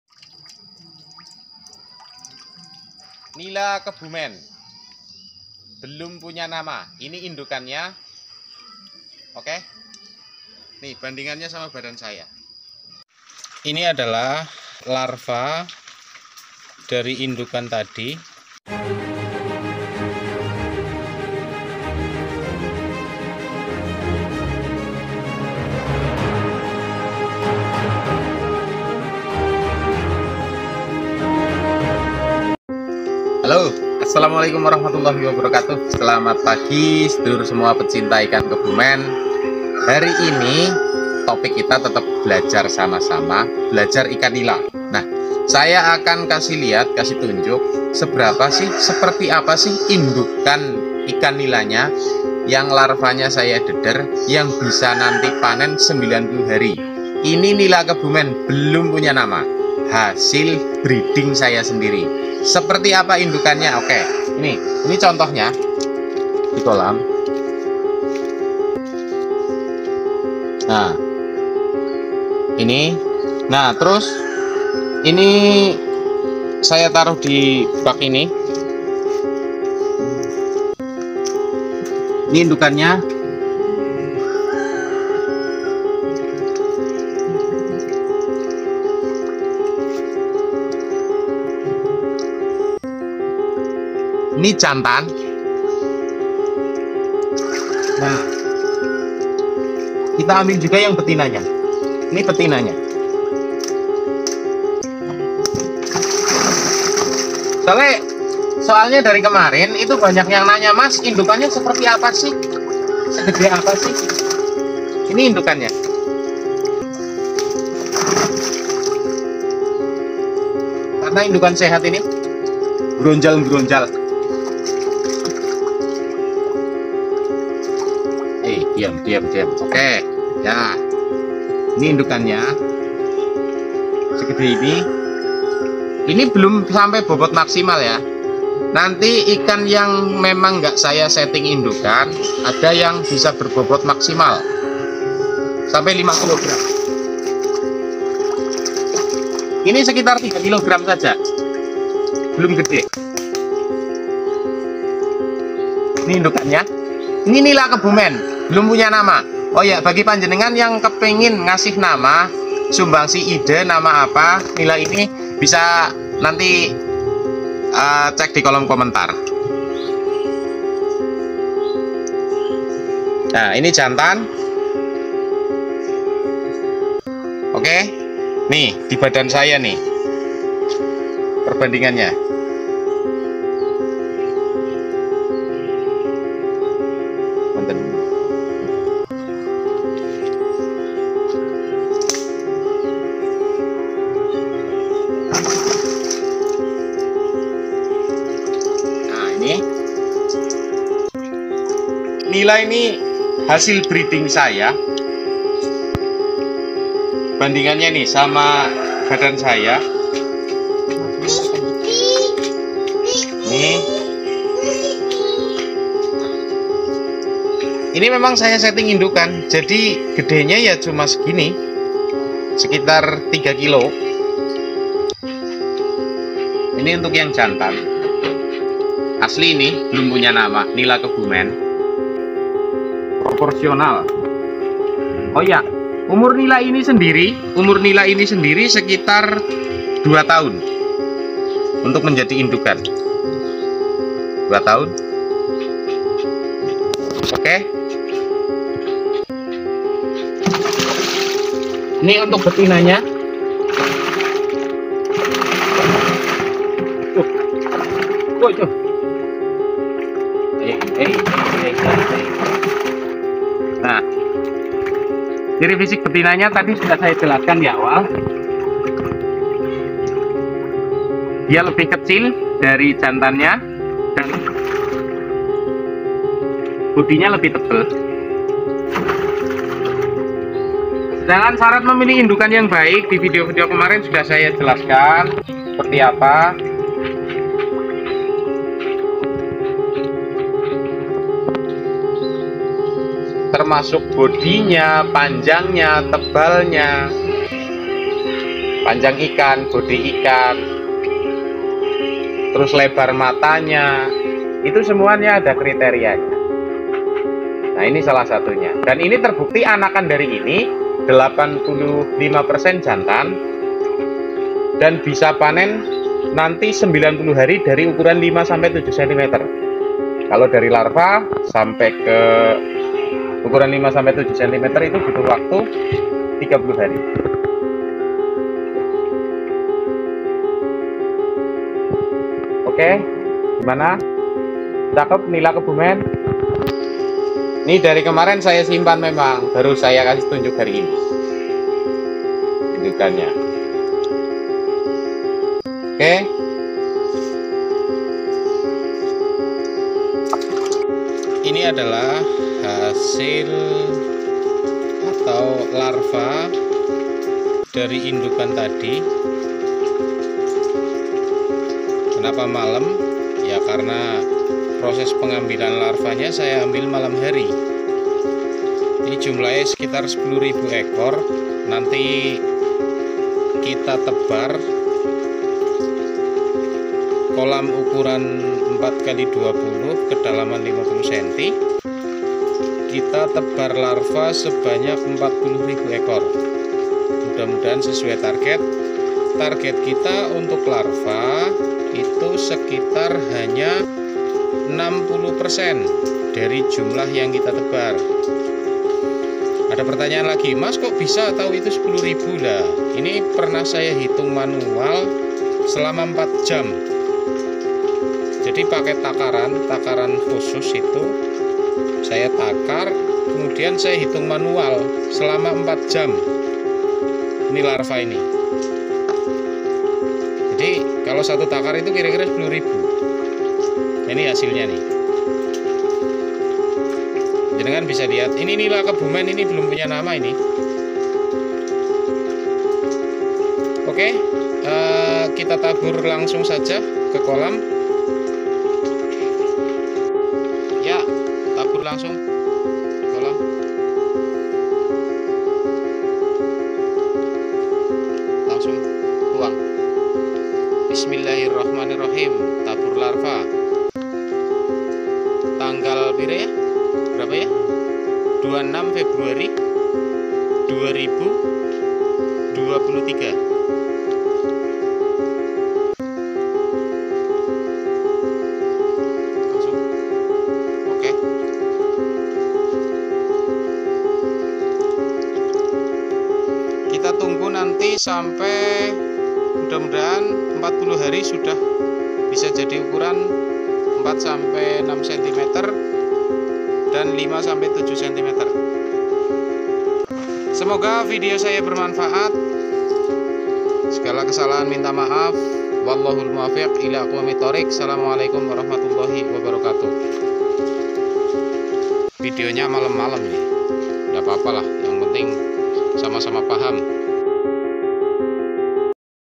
nila unggul kebumen,air asin jos air tawar jos
budidaya nila berilmu

cara menabur bibit nila yang benar.
kesalahan kecil tapi fatal akibatnya terutama pembudidaya ikan nila pemulau